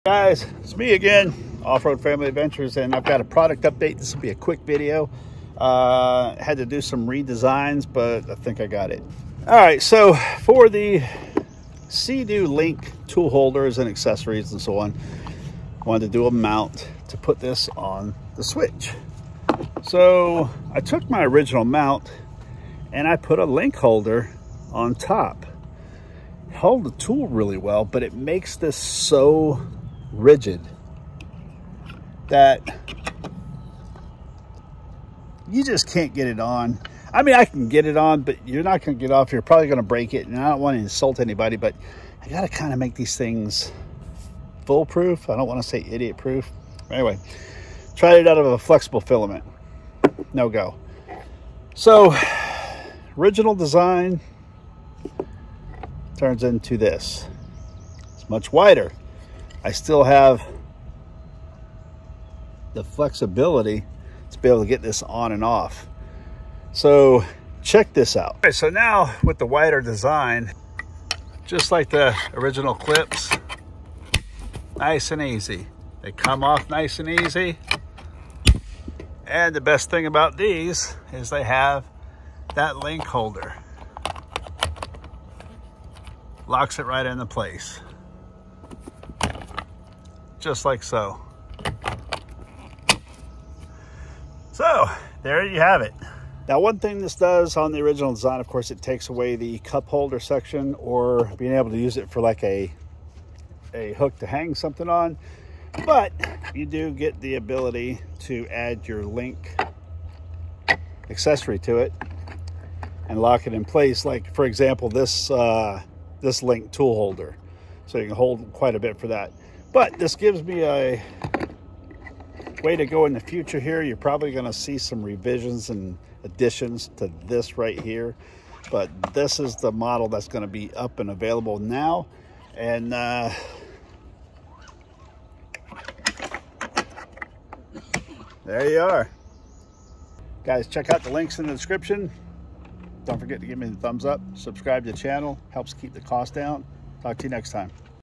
Seadoo switch clip with LINQ
New and better design for the Seadoo switch clip with a LINQ port.